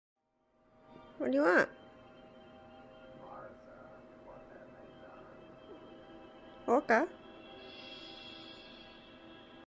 Oscar looking for some love!!
Here is Oscar aka Oka, my rescue cat demanding some attention!!